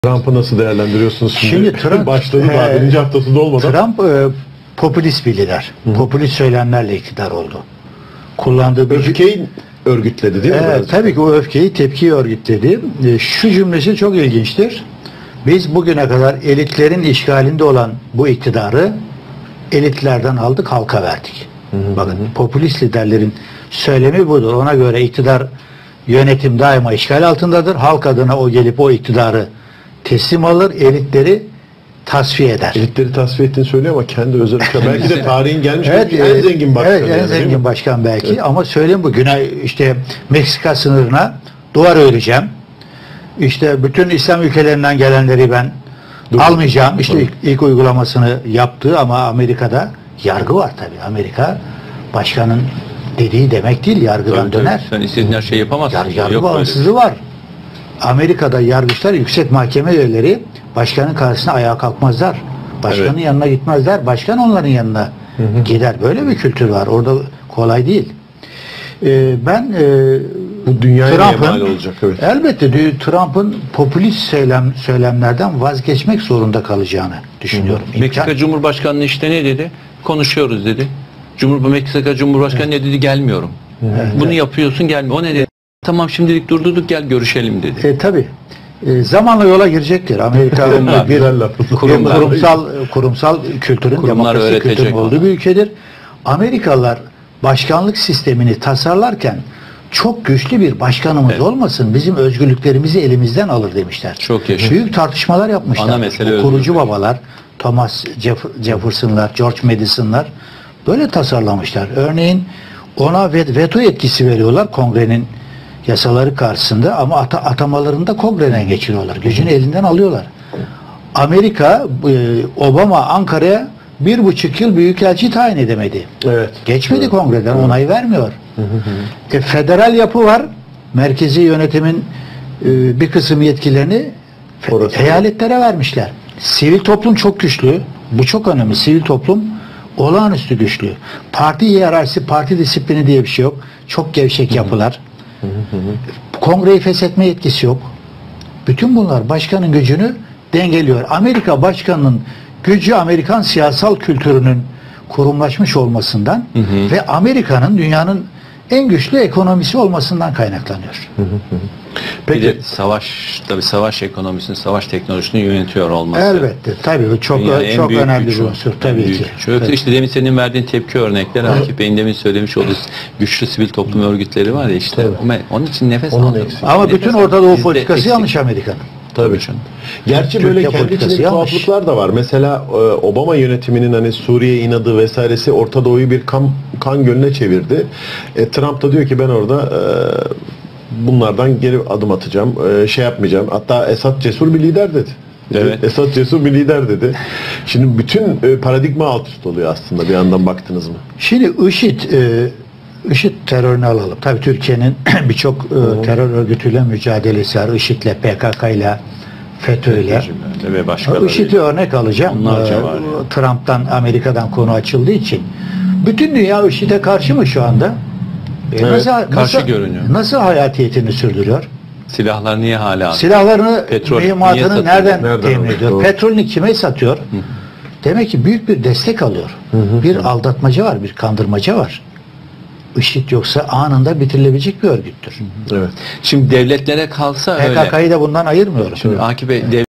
Trump'ın popülist söylemlerden vazgeçmek zorunda kalacağını düşünüyorum
29 Ocak 2017 Halk TV

M. Akif HAMZAÇEBİ
İstanbul Milletvekili
TBMM Başkanvekili